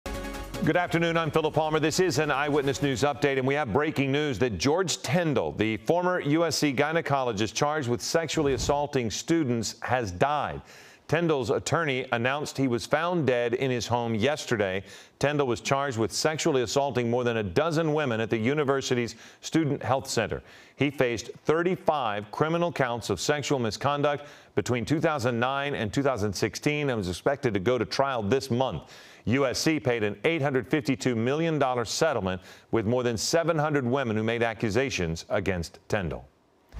George Tyndall, ex-USC gynecologist accused of sex abuse, found dead
Dr. George Tyndall, the former USC campus gynecologist accused of sexually abusing hundreds of women, was found dead Wednesday at his home in Los Angeles, his attorney told ABC News.